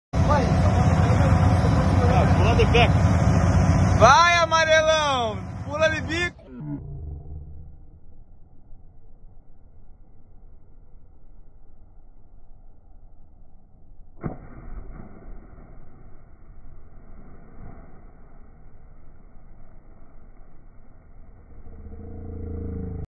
diver jumps out of the boat next to a cargo ship
diver jumps out of the boat next to a cargo ship on the atlantic ocean. diving operation